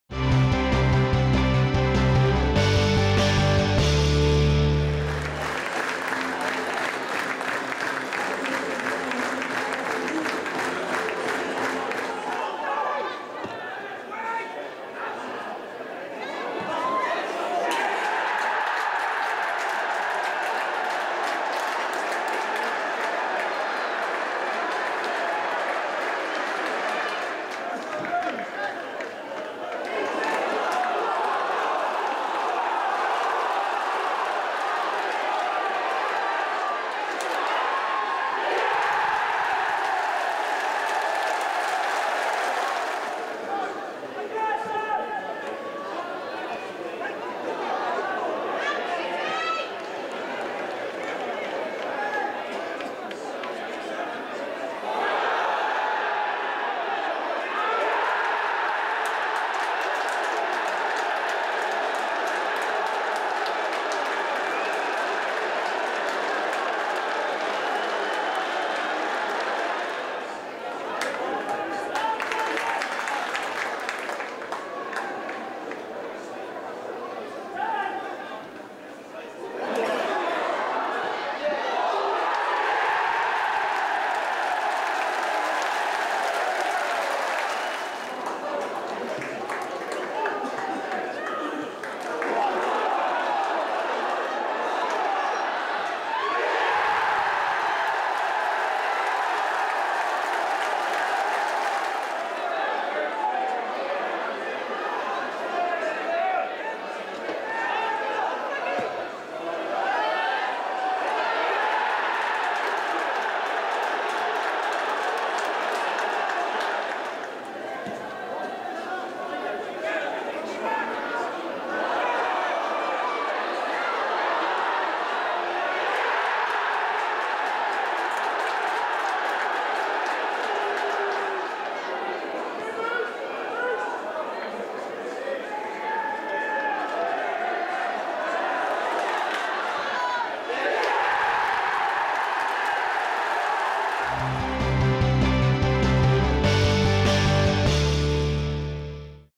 Coventry City Away Short Highlights
Short two minutes and 30 seconds of highlights from North End's eight goal thriller at the Sixfields Stadium, including North End's four goals from Tom Clarke, Bailey Wright - with an early goal of the season contender - Joel Byrom and Chris Humphrey with his first for the Club.